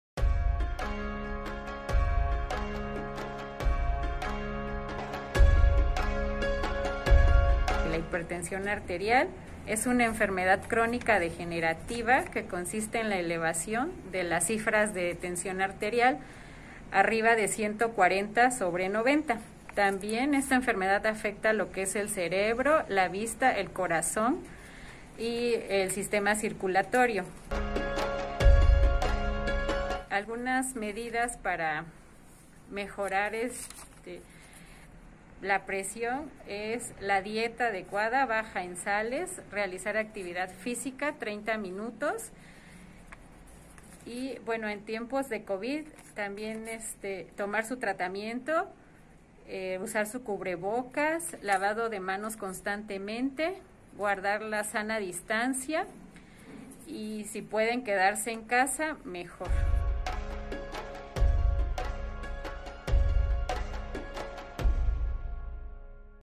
Cuidados: Hipertensión y COVID19
Para protegernos, proteger a nuestra familia y comunidad, debemos mantenernos informadas sobre los cuidados para disminuir los riesgos de contagio del COVID19.